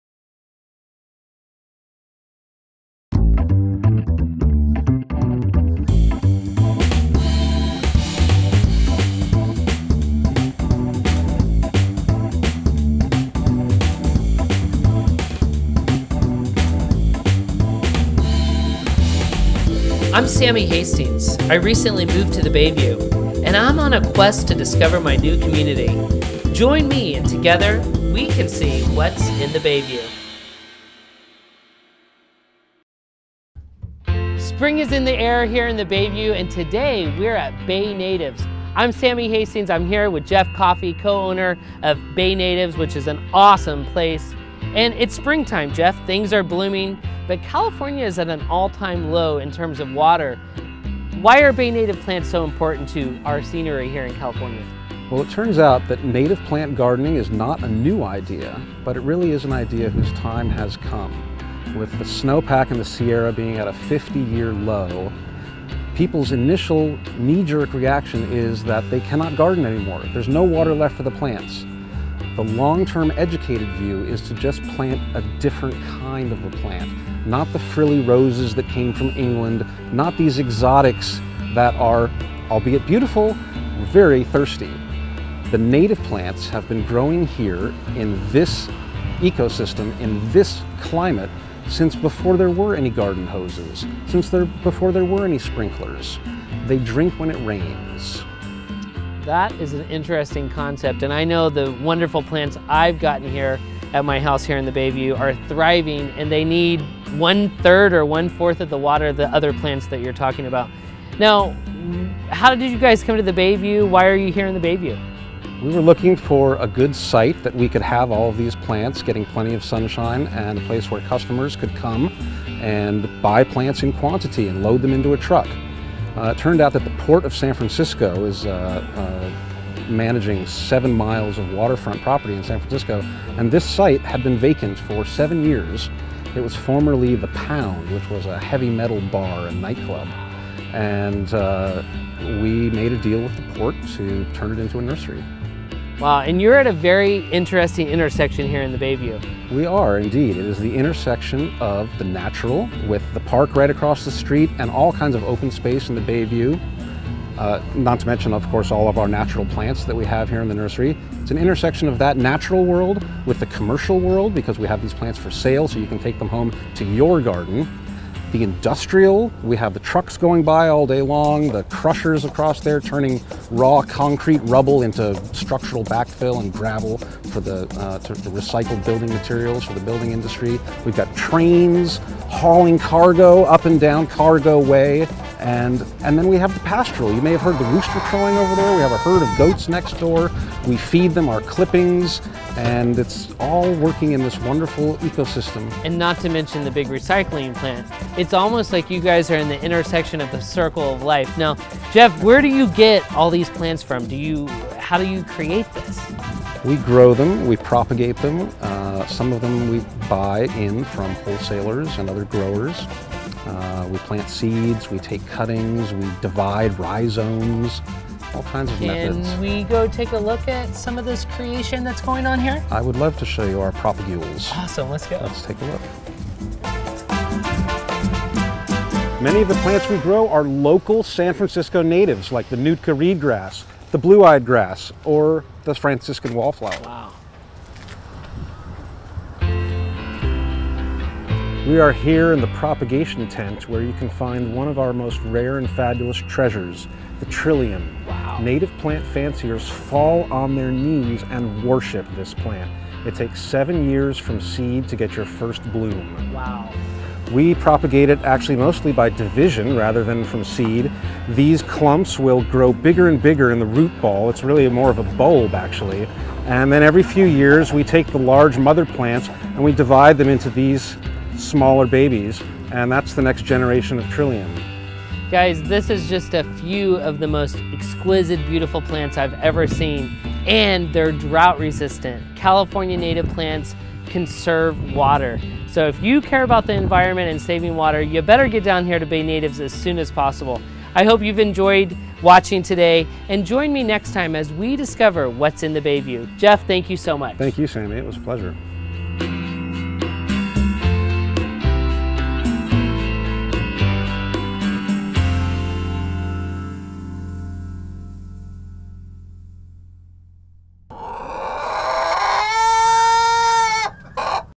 Bay Natives - Bayview - San Francisco
Bay Natives is recognized as the Bay Area's premier source for California native plants, offering distinctive and little-known native selections for the urban landscape. With a wide range of rare and endemic Bay Area locals, plus many choice specimens from across the state of California, they have inventory of more than 400 native plant species. Local San Francisco plants remain a specialty, but they also take pride in their extensive oak, manzanita, and trillium collections.
When founded in 2005, Bay Natives was an online business only, and have retained the shopping cart function on their website to this day. Inventory listed on the website is currently available in their store. Turnaround on most online orders will be 2-3 days; they can hold your plants for pick-up or deliver to your home or jobsite. If you have never taken a trip to this wonderful place get in the car and go straight there!